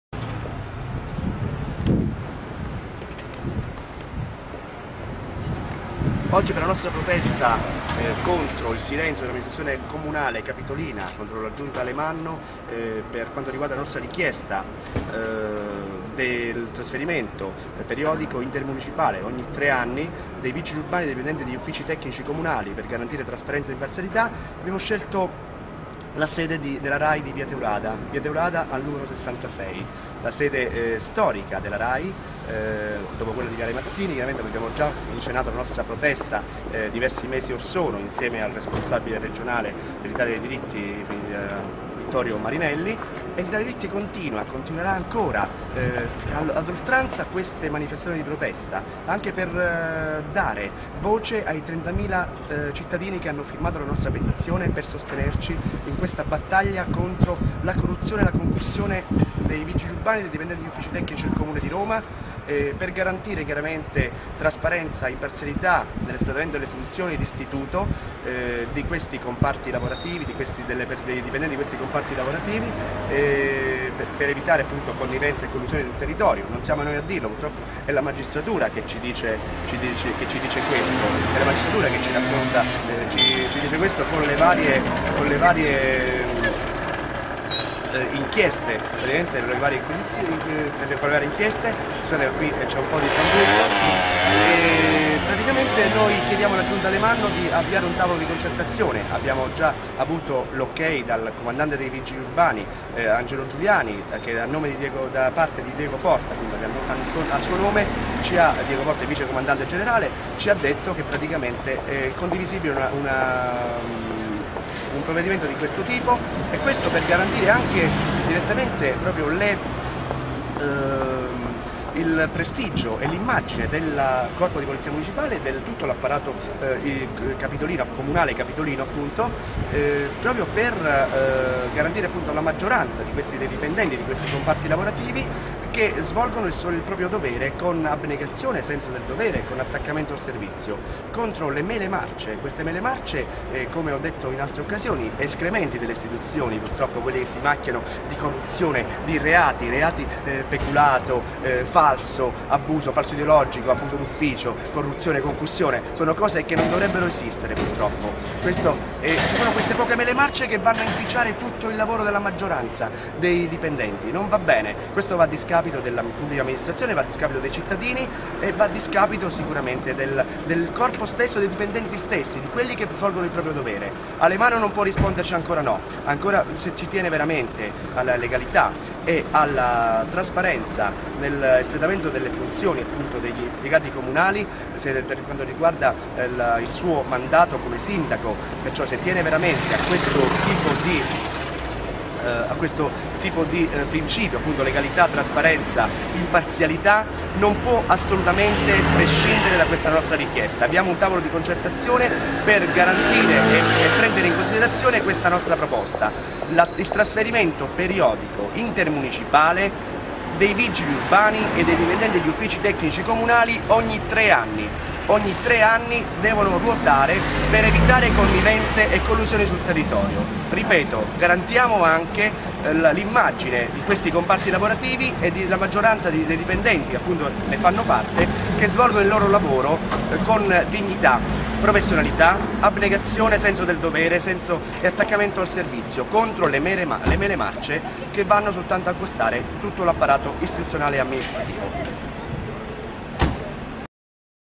Antonello De Pierro incatenato alla Rai di via Teulada contro corruzione vigili urbani
Il presidente dell'Italia dei Diritti: Andremo avanti con altre iniziative in caso di persistenza del silenzio della giunta di Alemanno

Roma Davanti alla sede romana della Rai di Via Teulada prosegue senza soluzione di continuità la serie di incatenamenti di protesta da parte del presidente dell'Italia dei Diritti Antonello De Pierro.

Motivo delleclatante gesto del fondatore e leader del movimento, la campagna contro la corruzione di alcuni, tra membri del Corpo della Polizia Municipale e dipendenti degli uffici tecnici comunali del Comune di Roma. Una problematica, sulla quale da tempo il movimento interroga le istituzioni, chiedendo la rotazione intermunicipale dei vigili urbani e degli impiegati delle U.O.T. dei Municipi. Duro lattacco di De Pierro alla giunta capitolina: Occorre prestare più attenzione nei confronti di un problema che oltre a ledere i diritti dei cittadini, minaccia limparzialità e la trasparenza nellespletamento delle funzioni della Pubblica Amministrazione, principi sanciti costituzionalmente dallarticolo 97. Numerose le iniziative portate avanti dallItalia dei Diritti di fronte al reiterato silenzio del sindaco Gianni Alemanno e alla mancata convocazione di un tavolo di discussione con le istituzioni. Trentamila le firme raccolte a sostegno della battaglia, mentre un apposito gruppo sul social network Facebook ha raggiunto circa quattromila iscrizioni. Dopo gli incatenamenti delle scorse settimane presso siti istituzionali e sedi di giornali e altre televisioni, il presidente De Pierro annuncia altre proteste non escludendo in caso di persistenza di silenzio e immobilismo da parte delle autorità competenti il ricorso a forme più radicali di lotta non violenta, per sradicare questo malcostume.